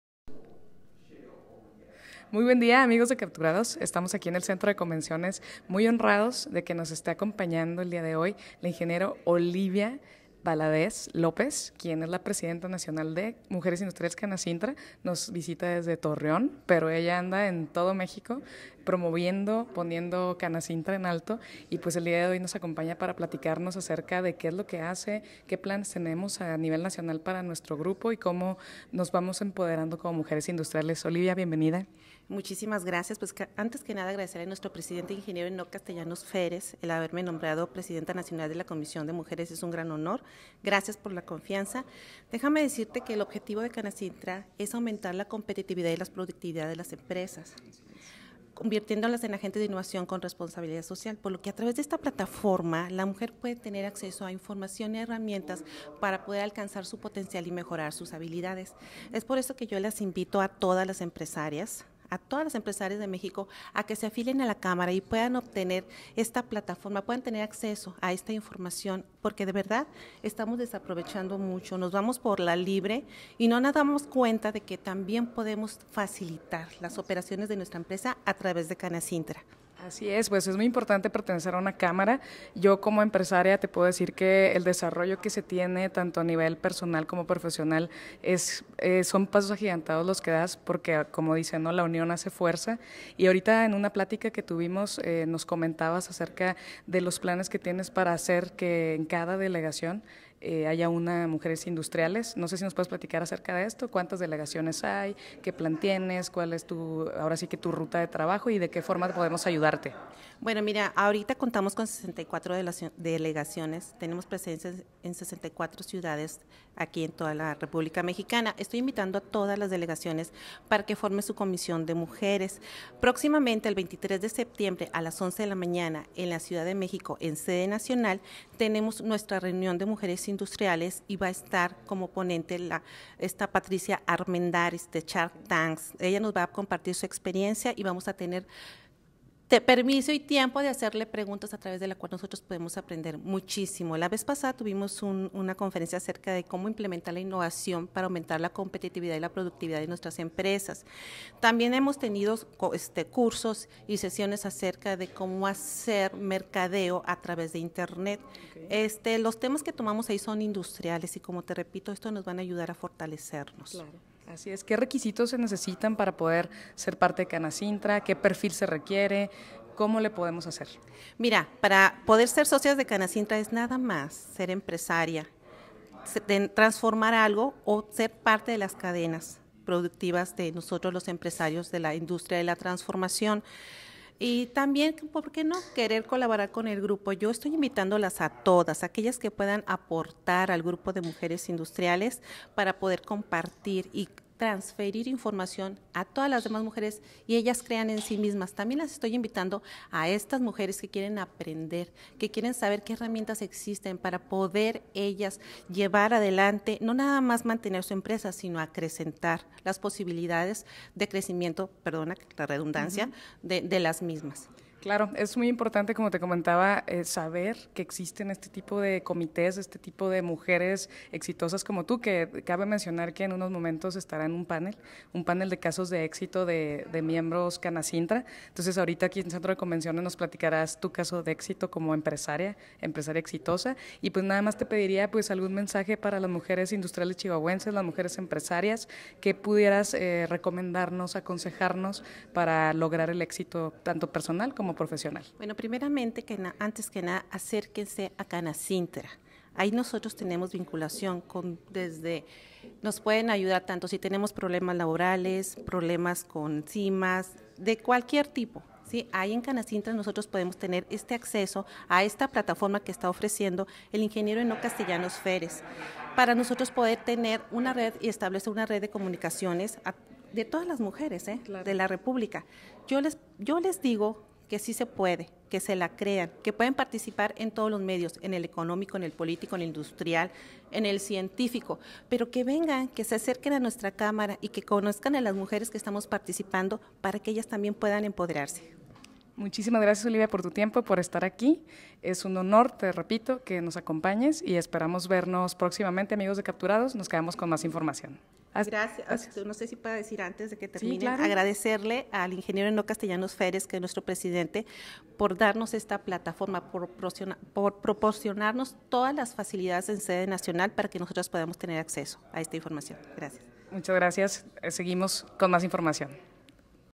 Canacintra chihuahua
Nos acompaña la Ing Olivia Valadez , Presidenta Nacional de Mujeres Industriales Canacintra en el evento Networking Regional Canacintra , acompáñanos en Centro de Convenciones